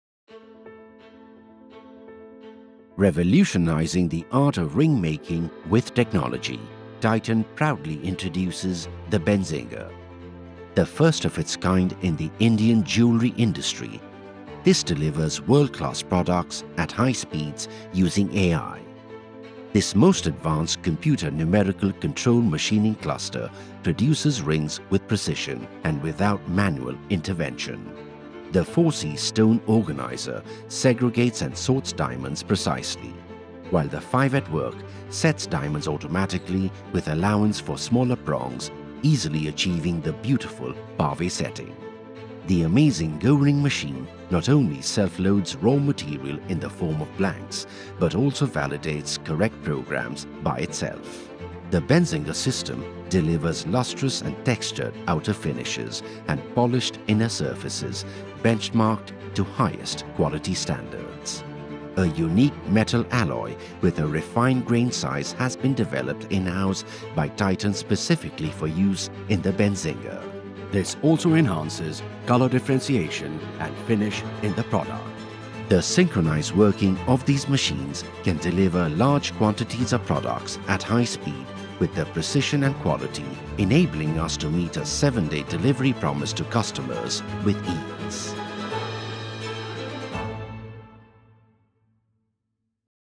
Revolutionizing Ring Making with Technology in Indian Jewellery Industry
Discover the revolutionary technology behind Titan's Benzinger. A new innovation in the ring making industry and the first in the Indian Jewellery industry; the system uses AI and advanced Computer Numerical Cluster (CNC) machining to produce precision rings without manual intervention.

Watch the video to see the future of ring-making!